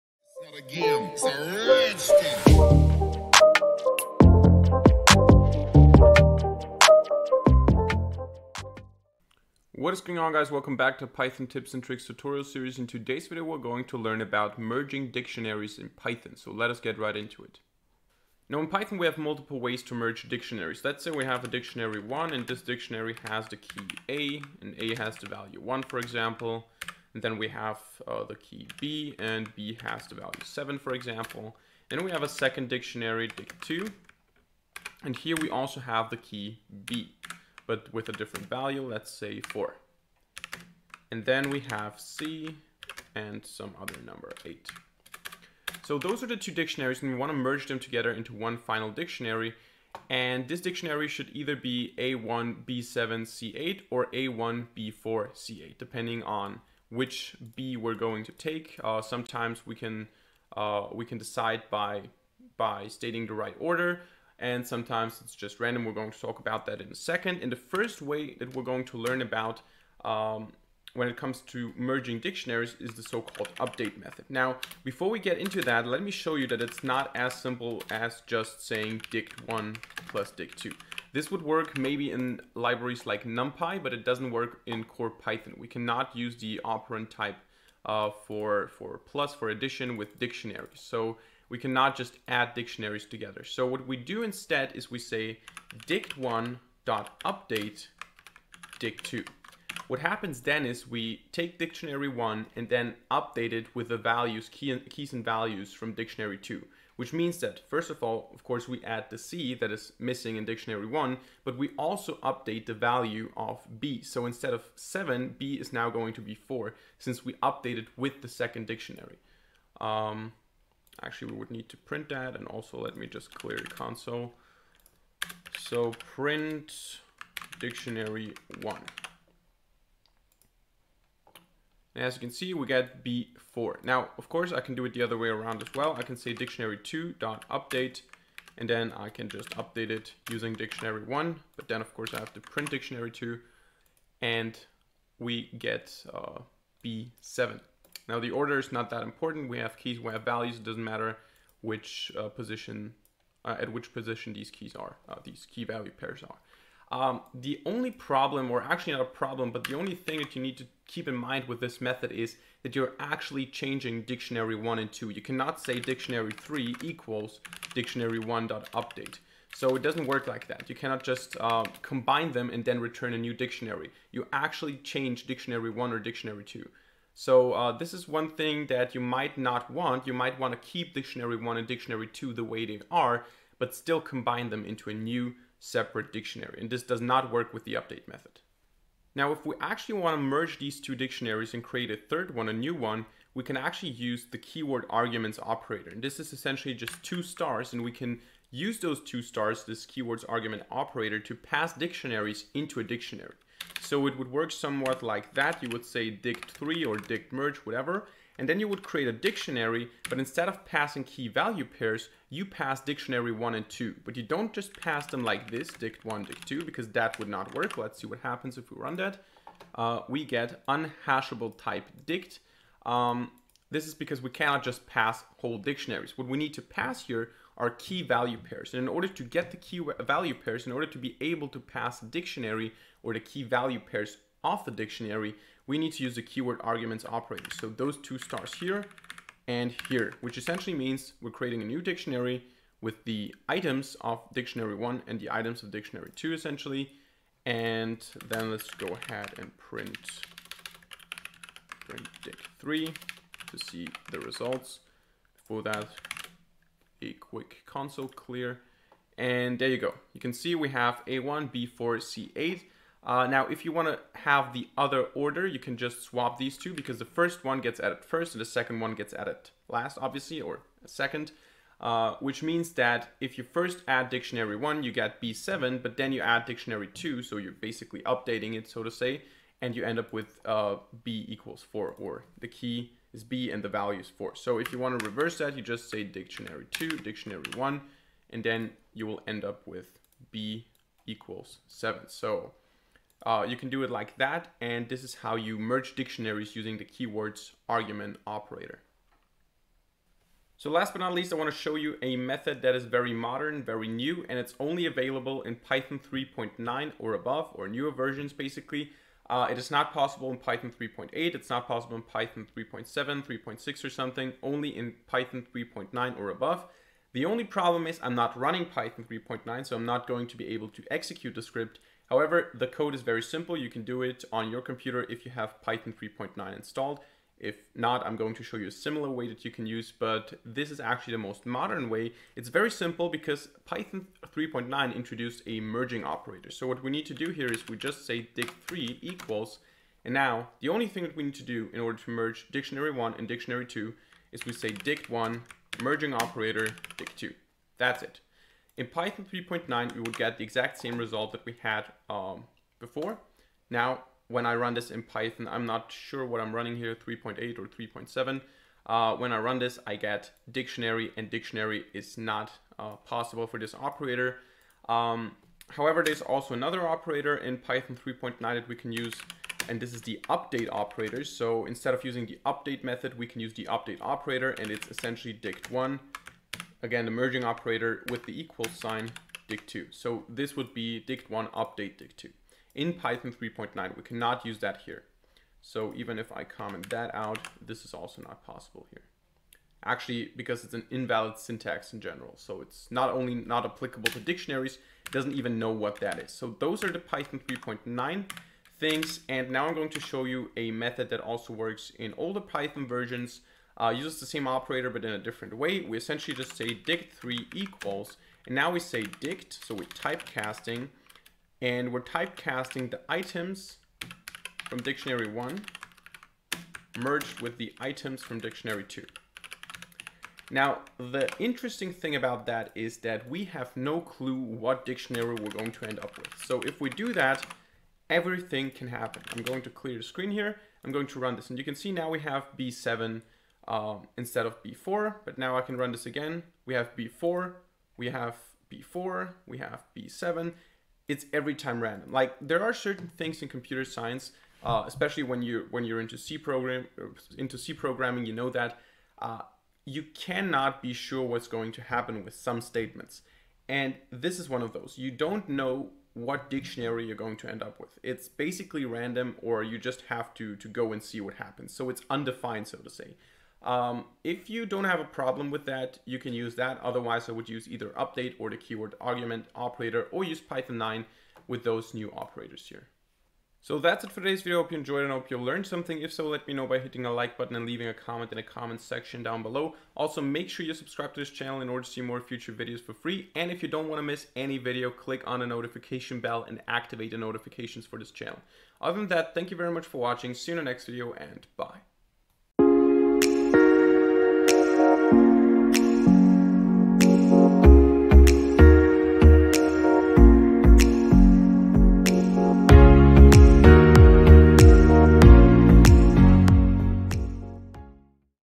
Merging Dictionaries - Python Tips and Tricks #13
In today's video we learn about different ways to merge dictionaries in Python.